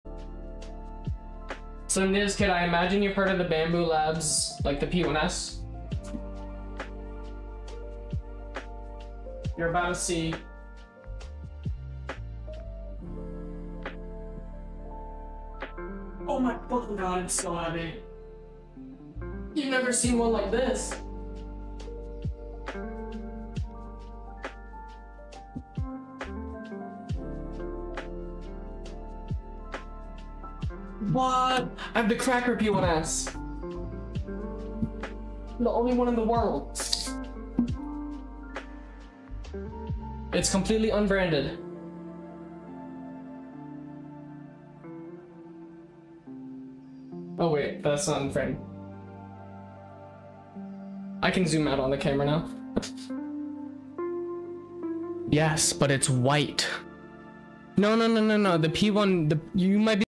Ava shows us her "3D Printer"
This is a clip from AvasLive's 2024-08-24 Twitch stream entitled HELP THIS FEMBOY MOVE INTO COLLEGE DORM!!!

This is a fan-made and uploaded video. AvasLive retains all rights to the stream content.